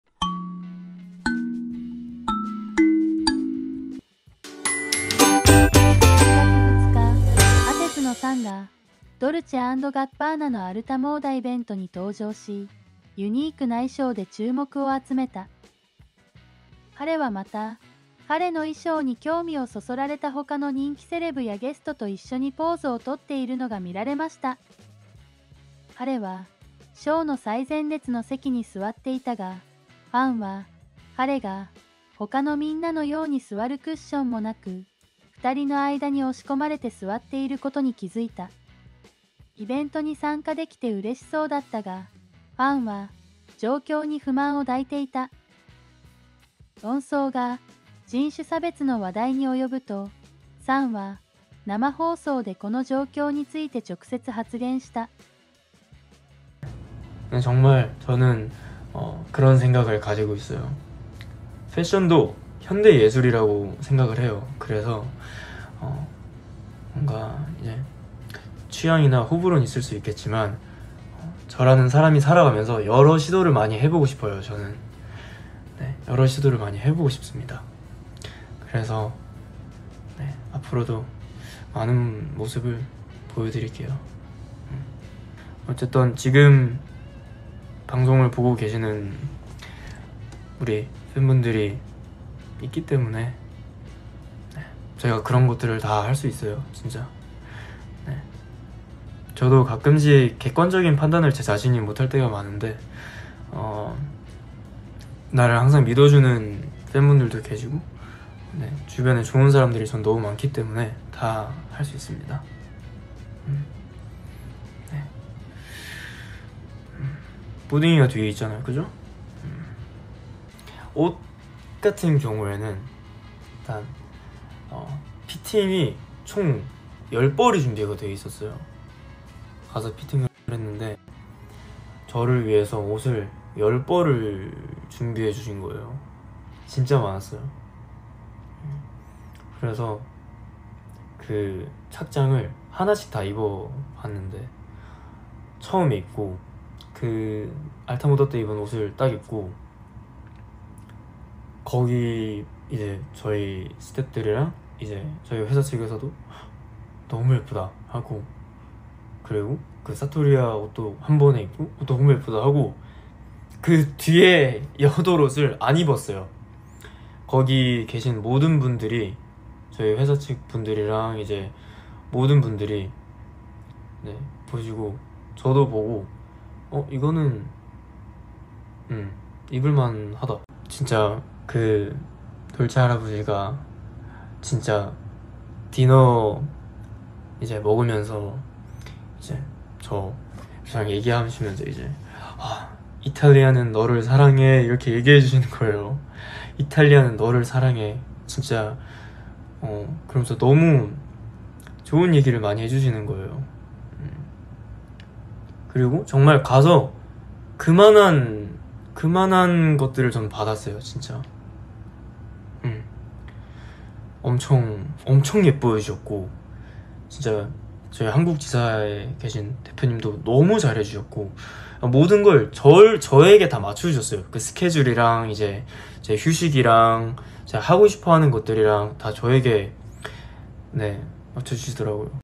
【ATEEZ】 ライブ配信するサン(SAN)。ドルチェ＆ガッバーナの物議を醸した衣装と人種差別の噂への反応。
【ATEEZ】 ライブ配信するサン。ドルチェ＆ガッバーナの物議を醸した衣装と人種差別の噂への反応。